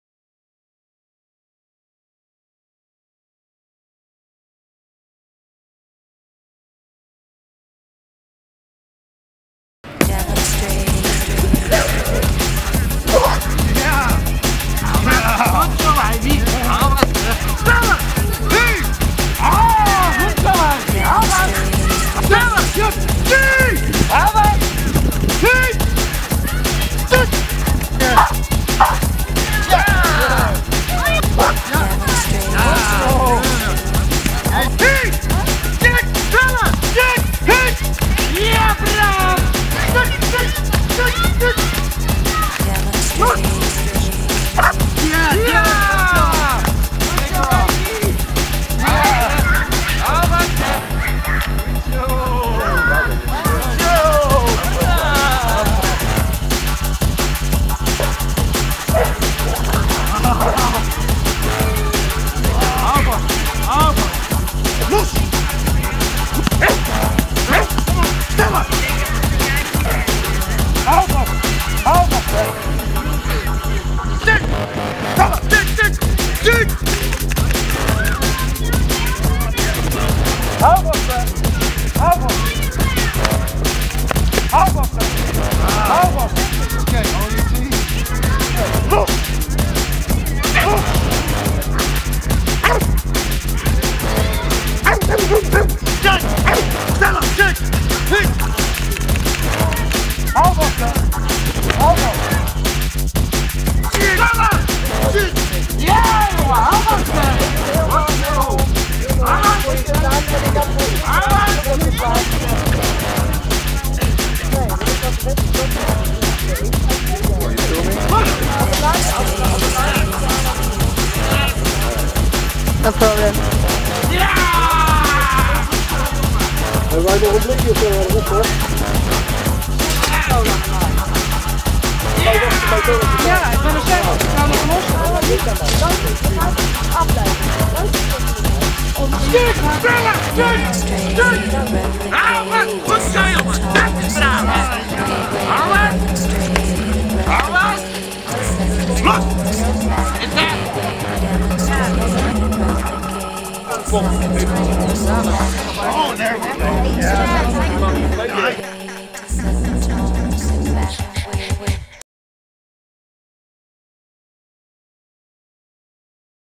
JOHN BONELLO WORKING WITH AMERICAN BULLDOGS AT EKC SHOW
John Bonello working with  American Bulldogs at EKC show  in Zeewolde, Holland. 2nd-3th of may 2015